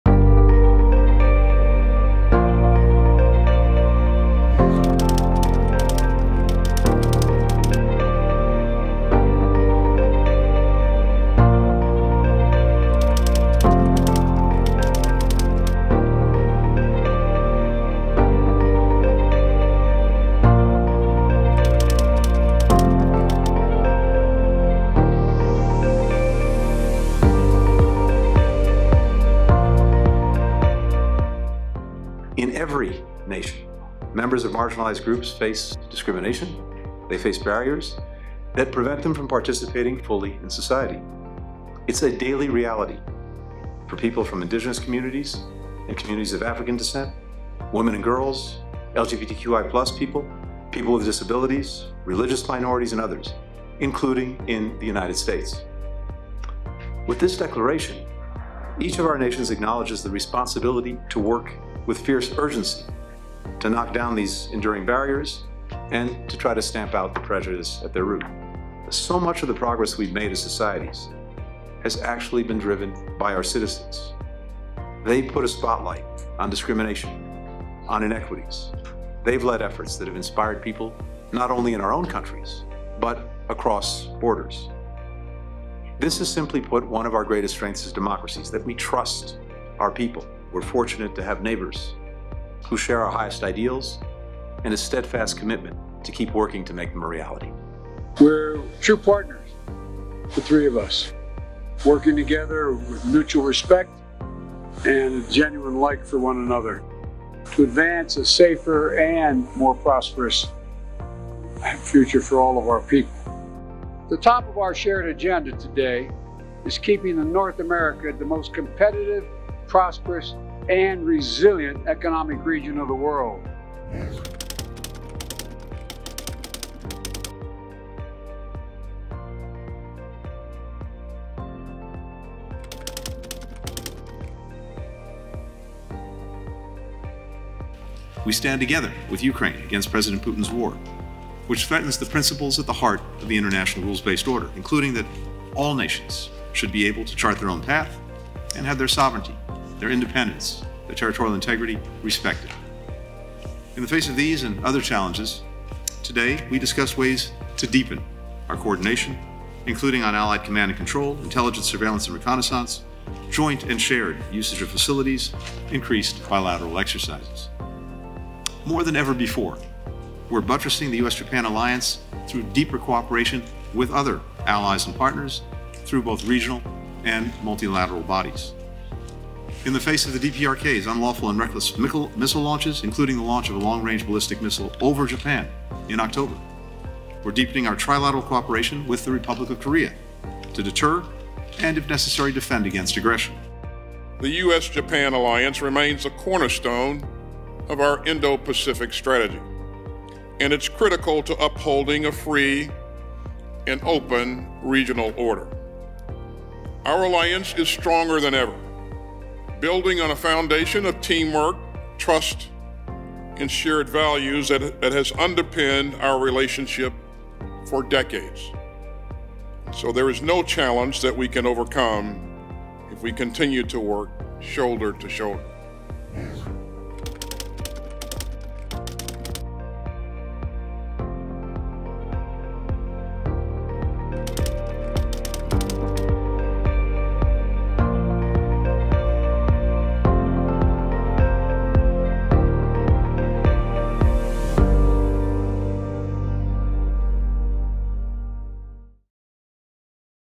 The Week At State • A review of the week's events at the State Department, January 7-13, 2023
In “The Week at State,” President Biden and Secretary Blinken travel to Mexico to attend the North American Leaders’ Summit, and Secretary Blinken and Secretary of Defense Austin co-host the 2023 U.S.-Japan Security Consultative Committee meeting.